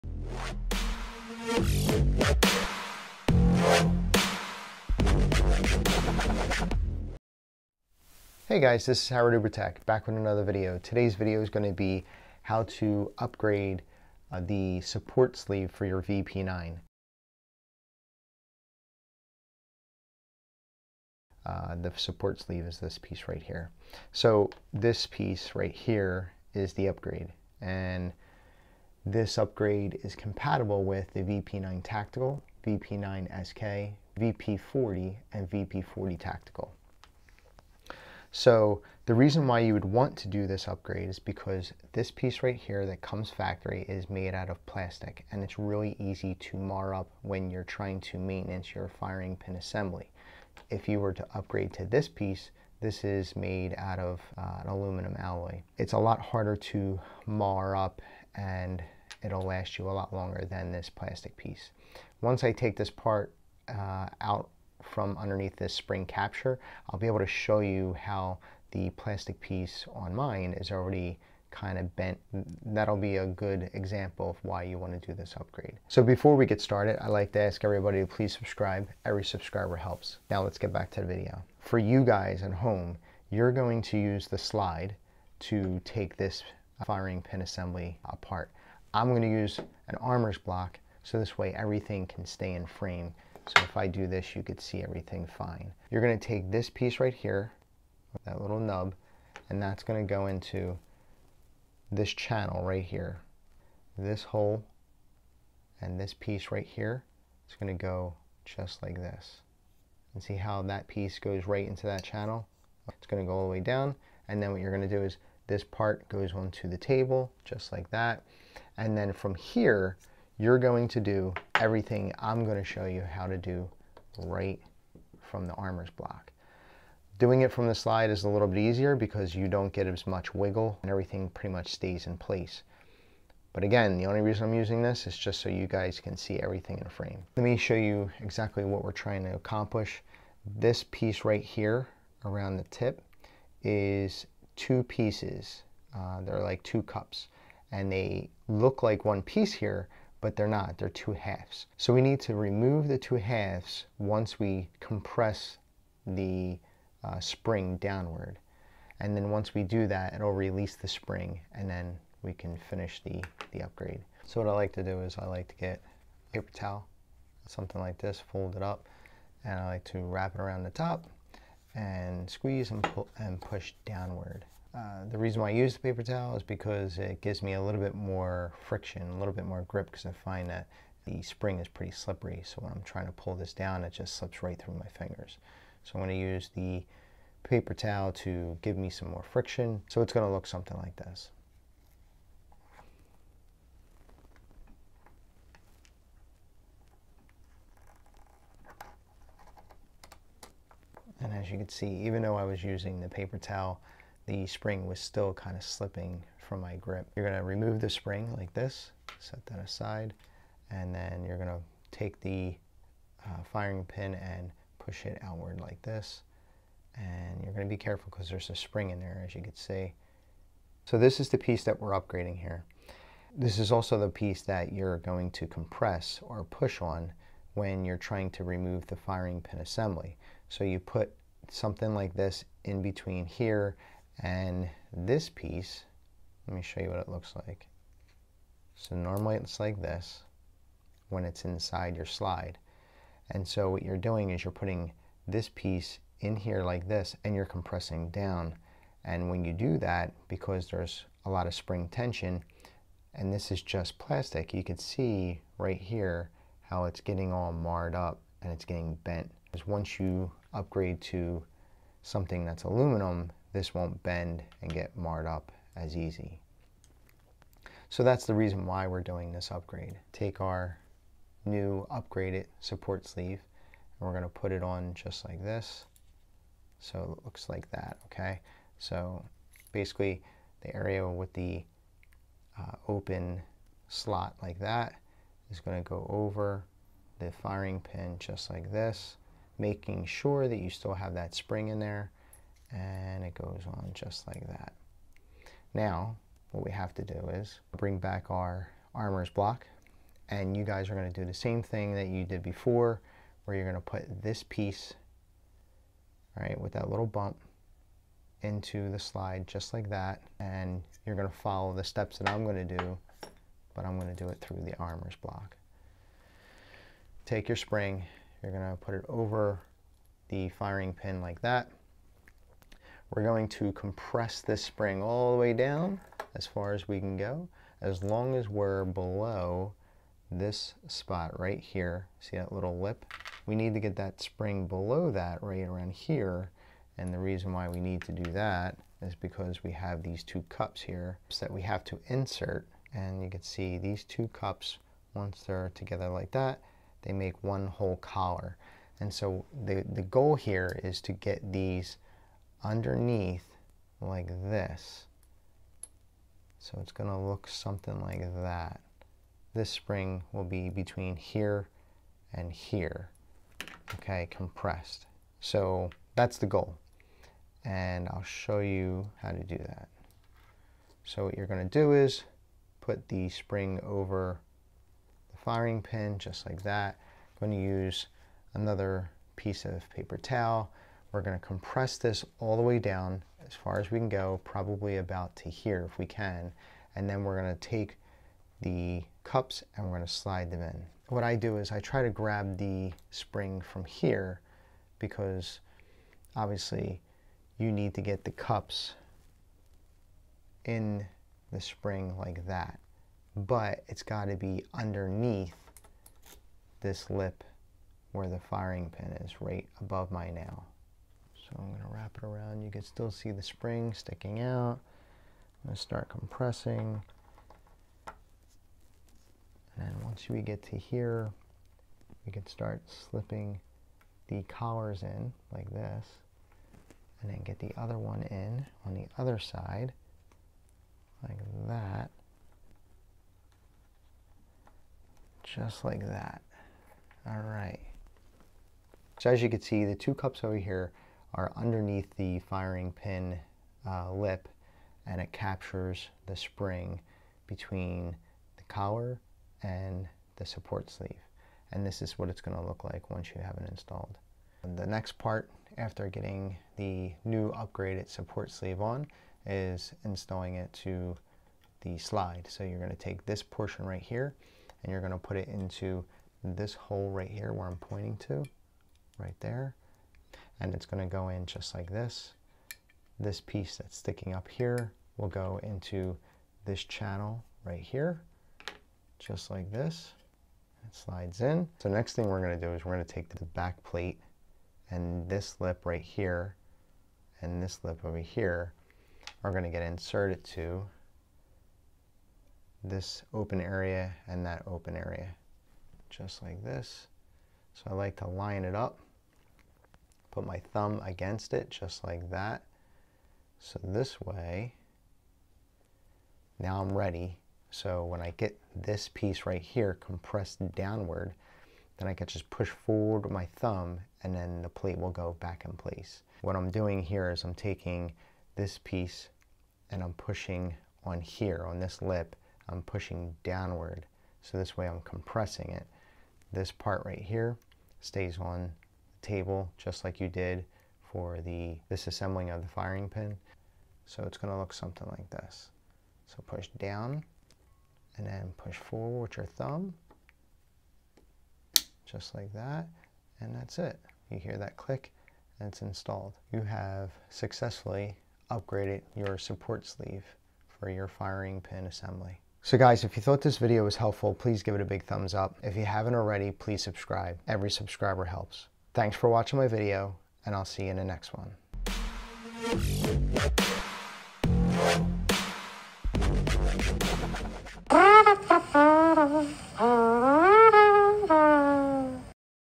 How to replace your Support sleeve for the VP9
Harry shows you how easy it is to replace your Support sleeve for the VP9.

This video is to show you how to assemble and disassemble purposes of repair or maintenance only.